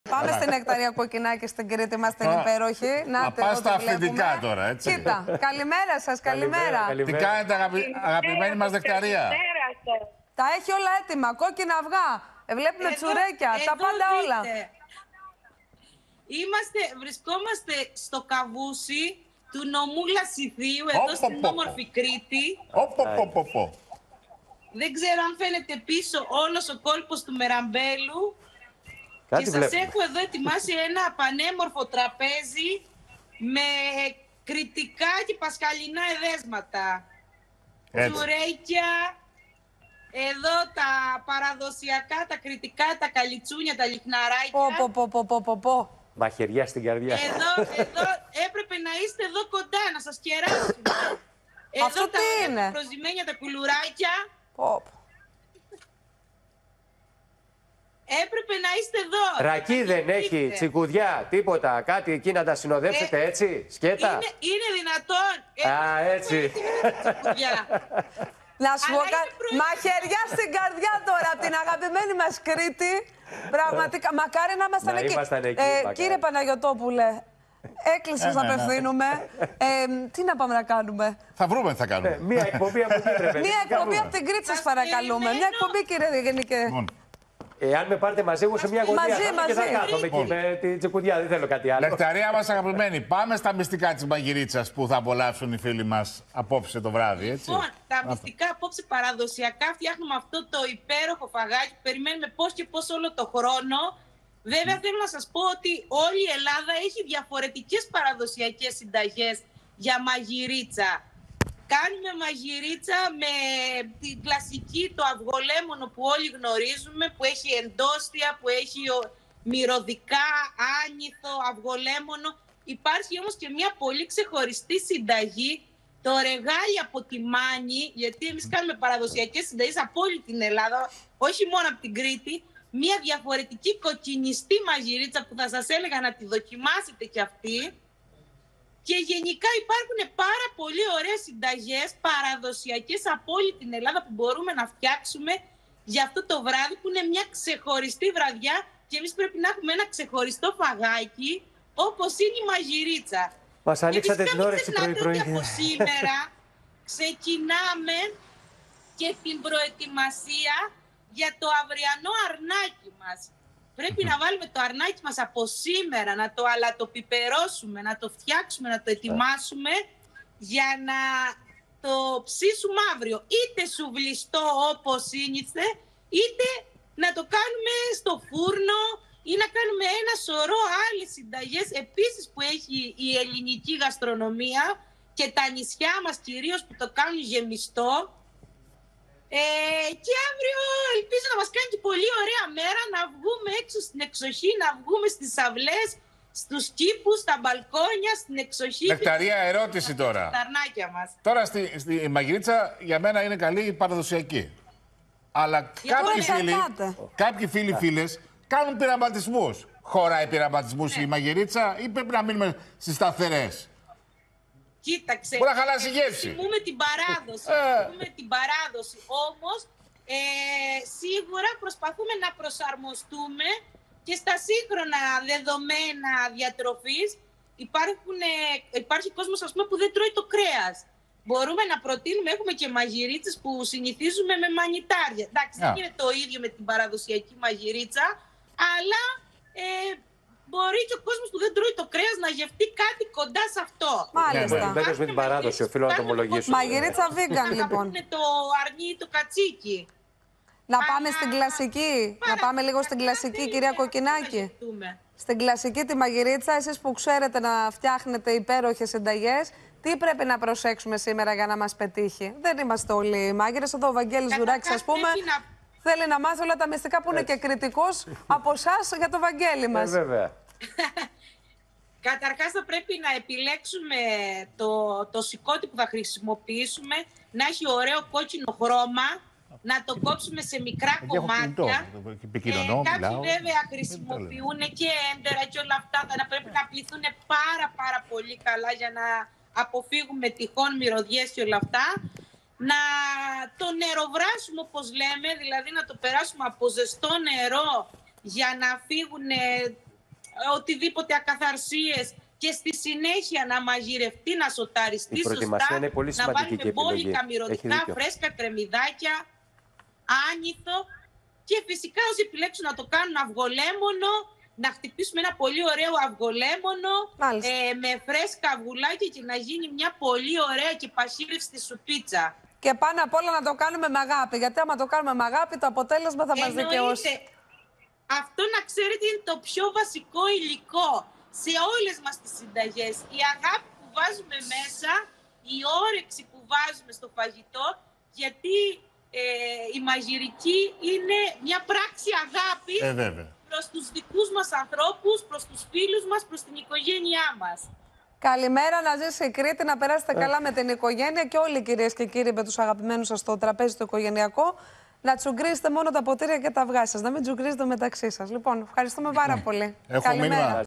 Η Νεκταρία Κοκκινάκη μας μυεί στα μυστικά της μαγειρίτσας και του αρνιού | OPEN TV
Ζωντανά από την Κρήτη βγήκε στην εκπομπή η Νεκταρία Κοκκινάκη και μας είπε τα μυστικά της για την μαγειρίτσα και το πασχαλινό αρνί.